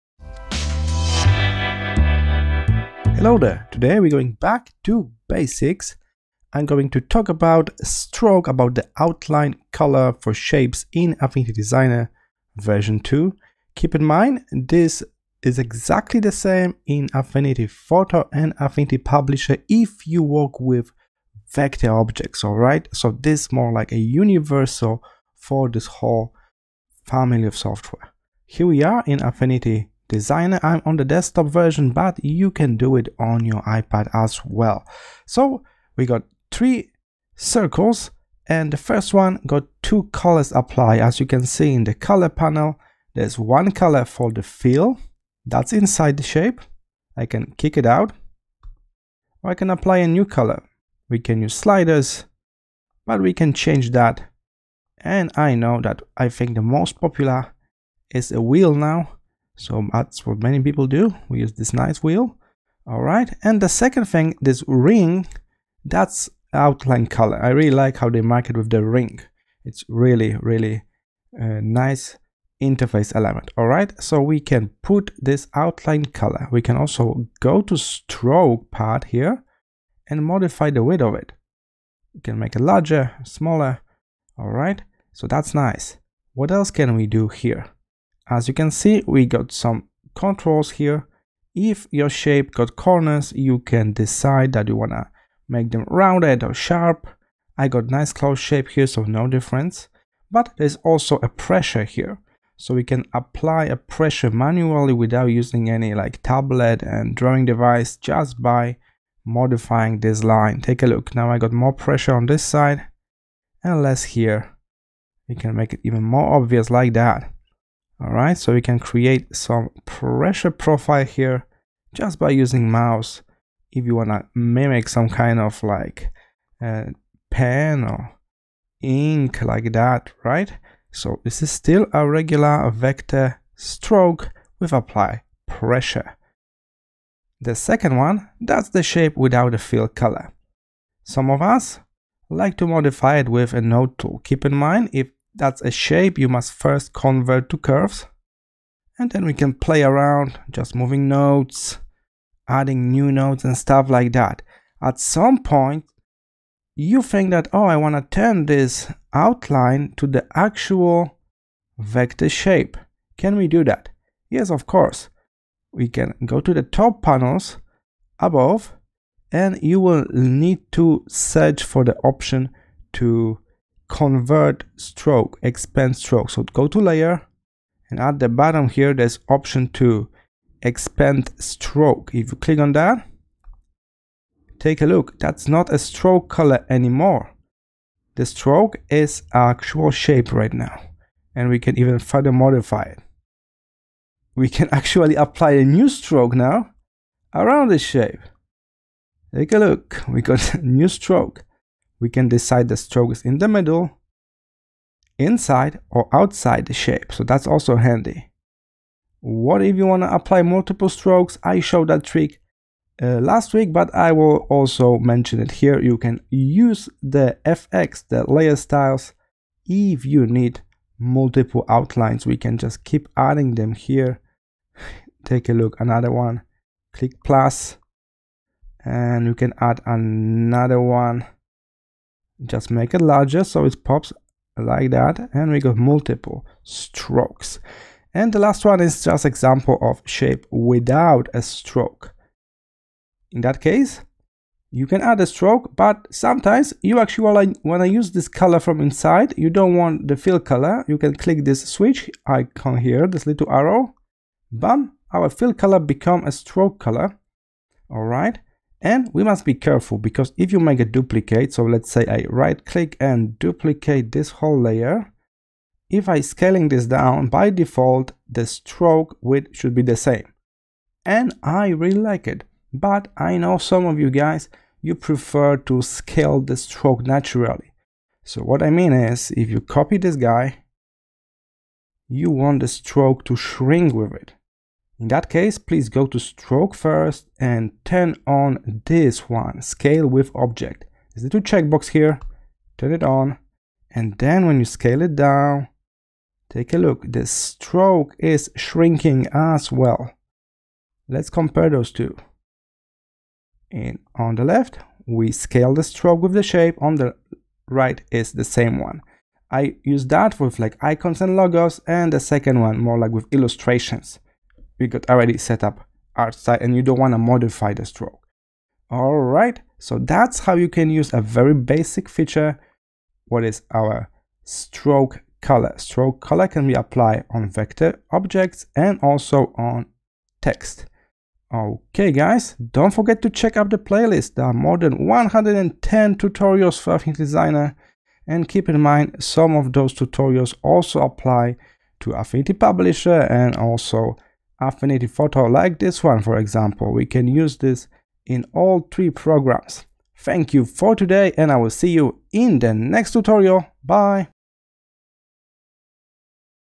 Convert Stroke to Shape in Affinity Designer 2
How do I convert a Stroke to a shape in Affinity Designer?
Select Layer and click Expand Stroke.